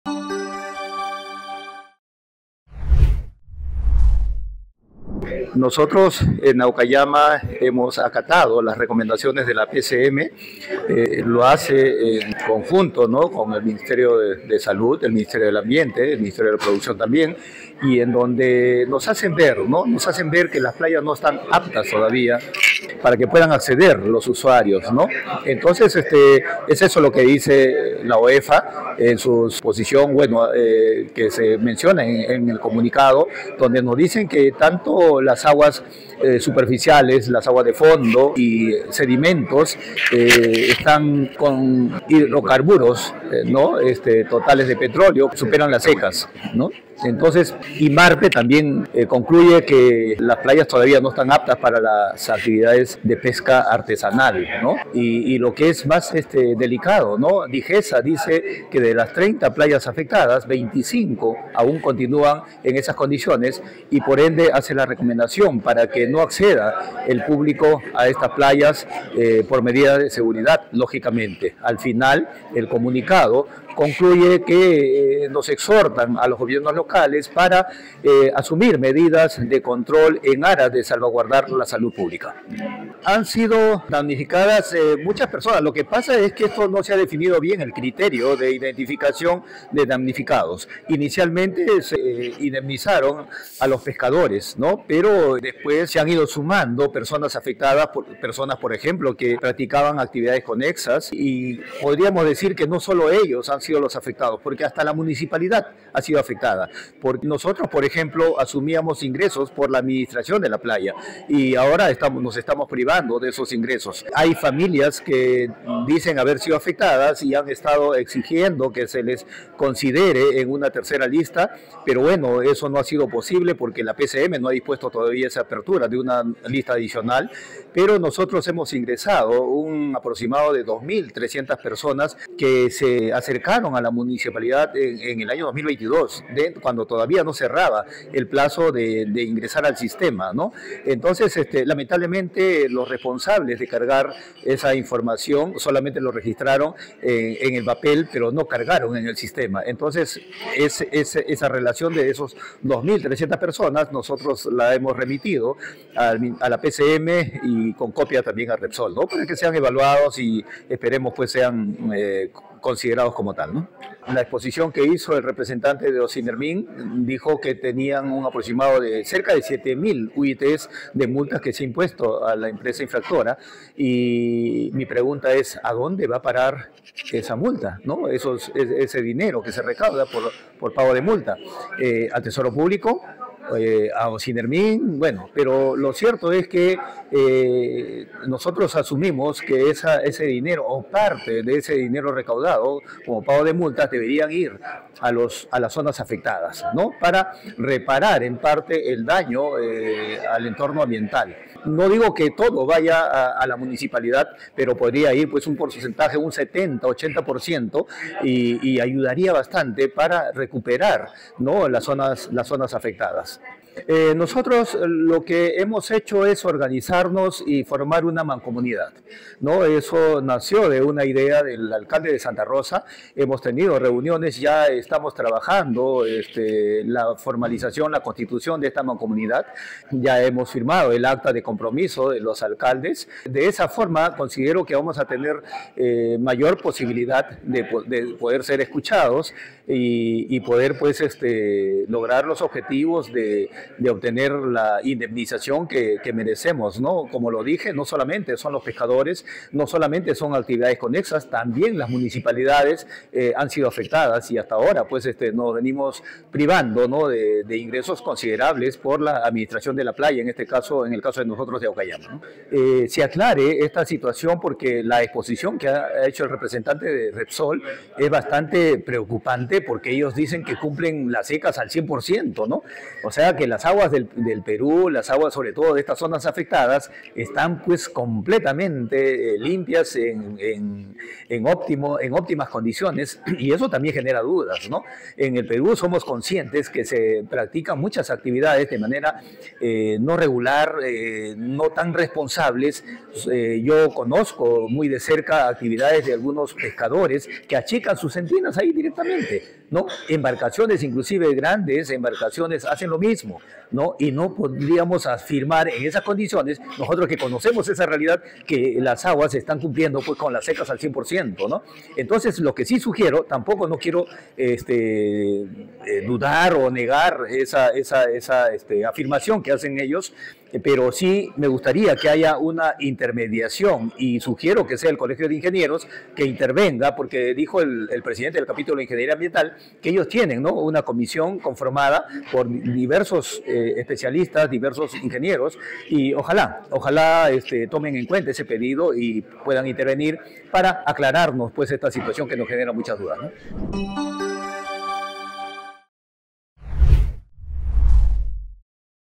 Foro: Derrame de petróleo en Ventanilla - Edwin Valdivia, Alcalde de la Municipalidad de Aucallama
"Foro Internacional: A dos años del DerrameDePetróleo en el Mar de Ventanilla" Edwin Valdivia Alcalde de la Municipalidad de Aucallama, provincia de Huaral, Región Lima